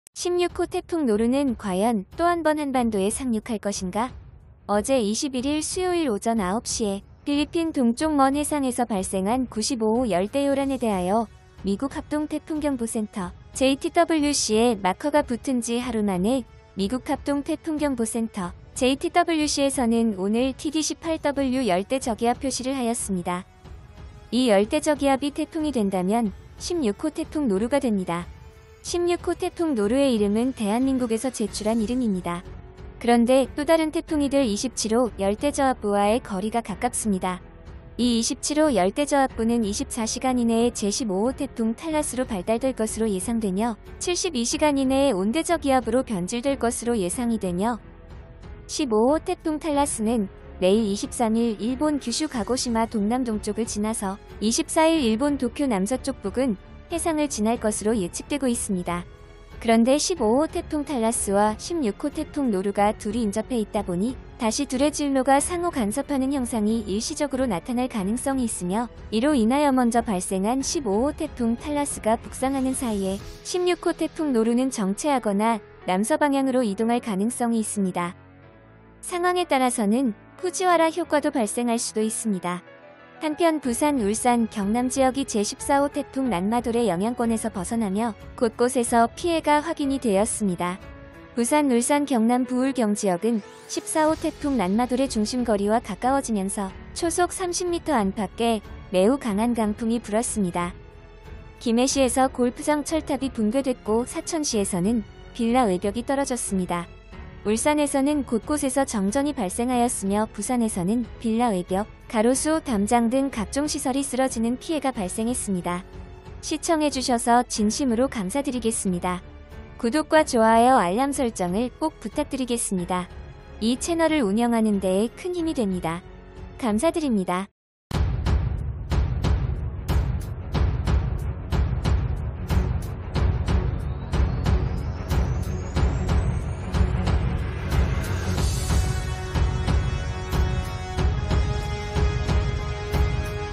15호태풍 탈라스 일본 도쿄관통 15호+16호 태풍 후지와라 효과 16호 태풍 '노루' 한반도 오는가?
15호 태풍 탈라스와 16호 태풍 노루가 둘이 인접해 있다 보니 다시 둘의 진로가 상호 간섭하는 형상이 일시적으로 나타날 가능성이 있으며 이로 인하여먼저 발생한 15호 태풍 탈라스가 북상하는 사이에 16호 태풍 노루는 정체하거나 남서방향으로 이동할 가능성이 있습니다